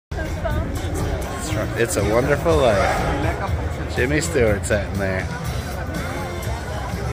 George Bailey’s 1919 Dodge phaeton from It's A Wonderful Life Celebration Exotic Car Festival
George Bailey’s 1919 Dodge phaeton from It's A Wonderful Life Celebration Exotic Car Festival jimmy stewart christmas movie